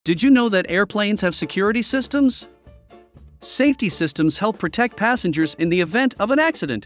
How are passengers protected in plane crashes?
Aircraft are equipped with a variety of safety systems that help protect passengers in the event of an accident. These systems include seat belts, crash seats, and oxygen masks. Seat belts help keep passengers in their seats in the event of an impact. Impact-resistant seats are designed to absorb impact energy and protect passengers from injury. Oxygen masks help passengers breathe in the event of a loss of cabin pressure.

In this video, you'll learn more about airplane safety systems and how they help protect passengers in the event of an accident.
air safety
air accidents
security systems
seat belts
impact proof seats
oxygen masks